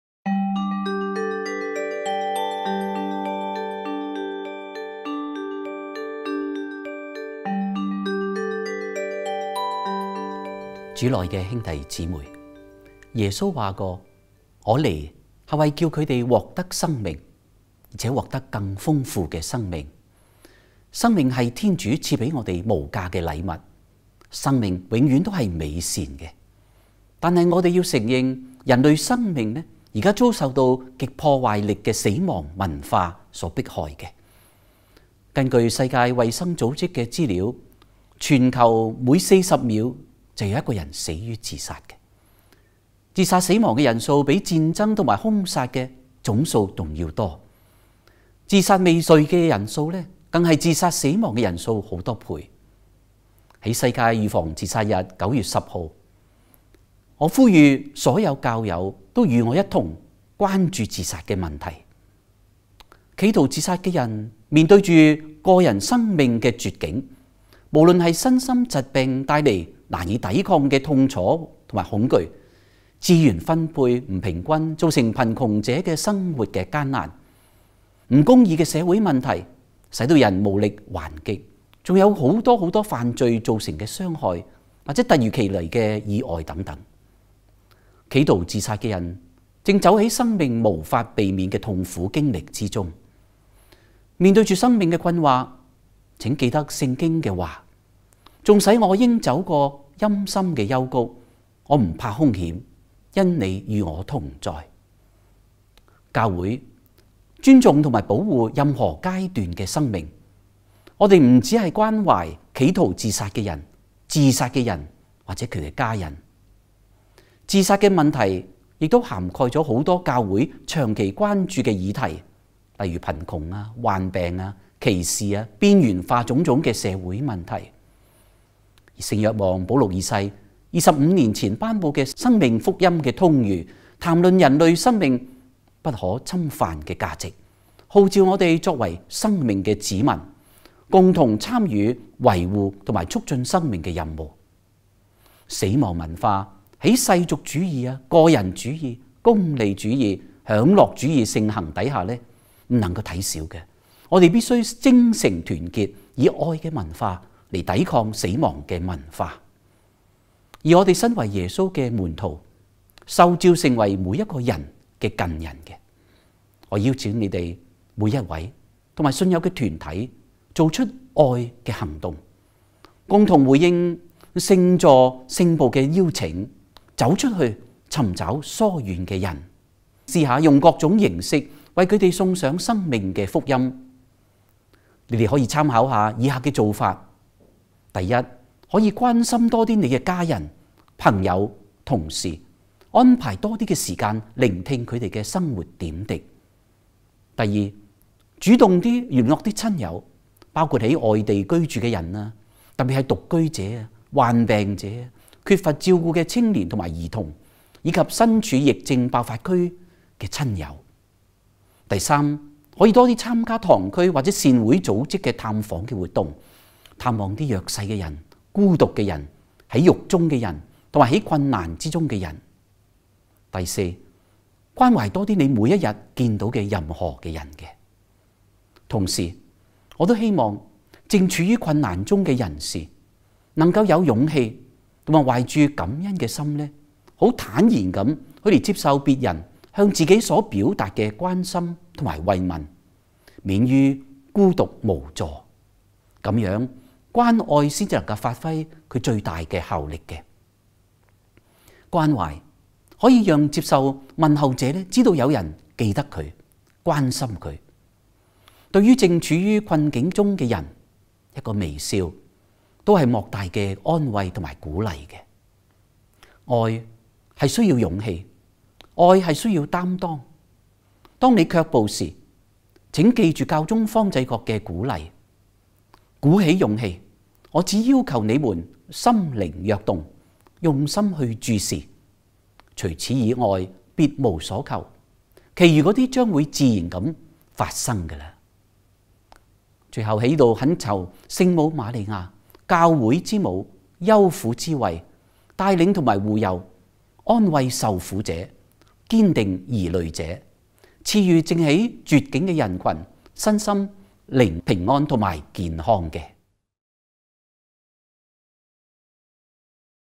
李斌生主教為響應世界預防自殺日文告(10/09/2020)
澳門教區社會傳播中心製作
MMXX